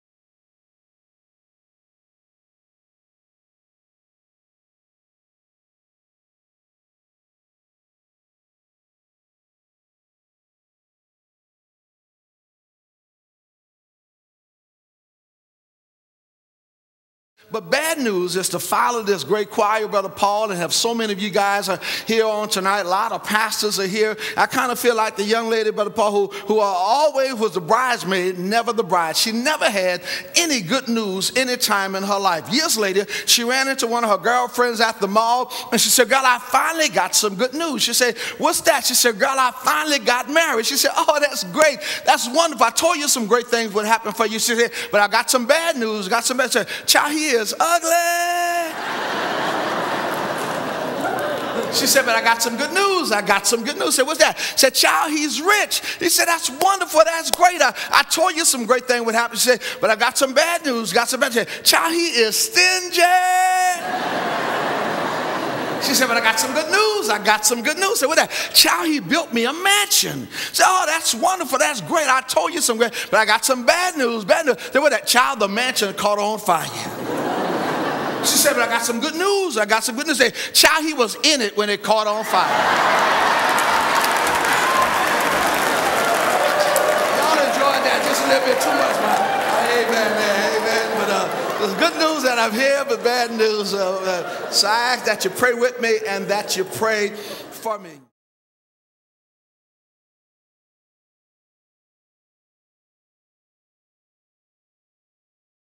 Fred Luter has Good News and Bad News
One of the opening stories that Dr. Fred Luter told at Northcrest Baptist Church on Sunday Night, April 25, 2010